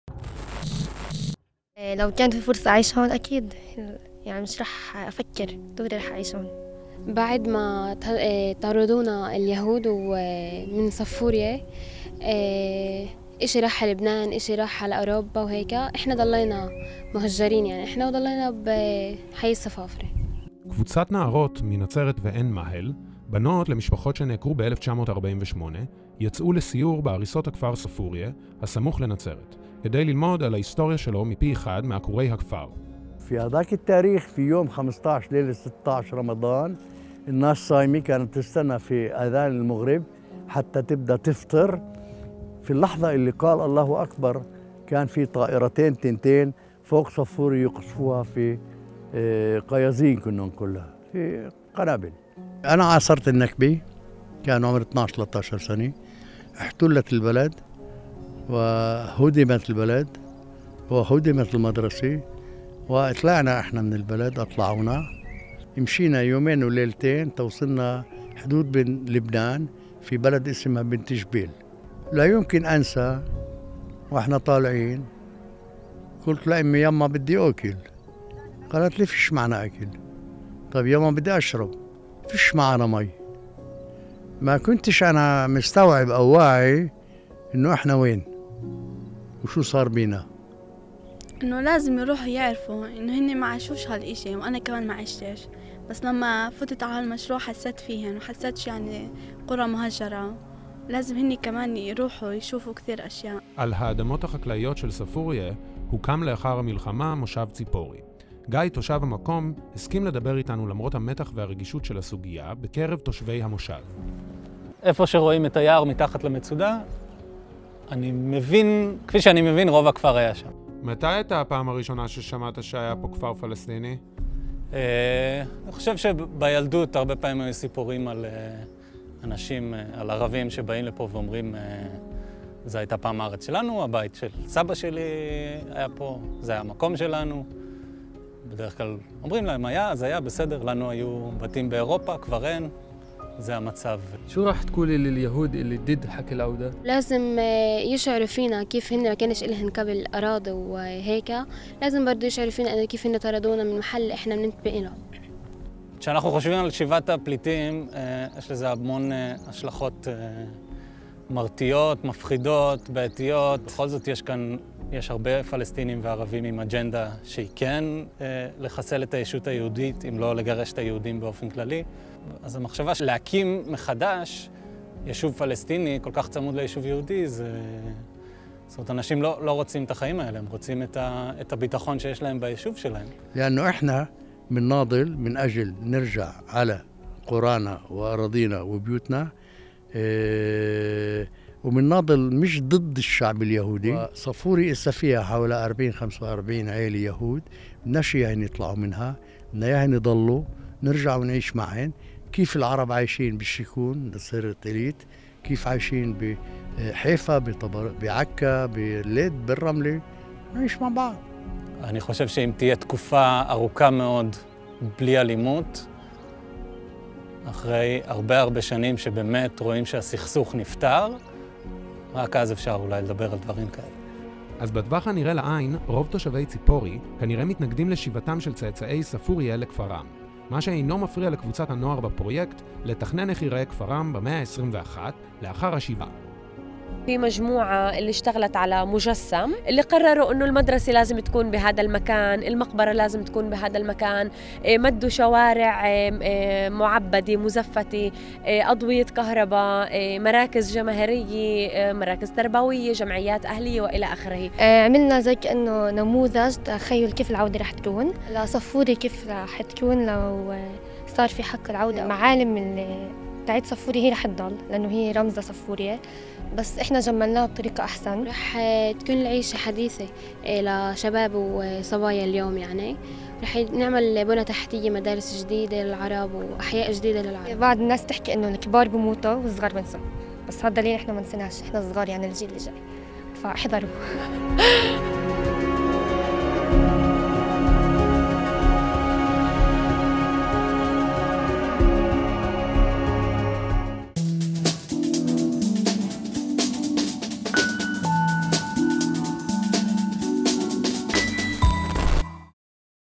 The return to Safuria
טיוטא"We are fighting for our return to our villages and our lands and our homes, we do not want them to leave, we want them to stay, to return and live together with them." Amin Muhammad Ali was born in Safuria (today the Zippori region) and dreams, together with a group of young people from the village, how the return to their land will look like.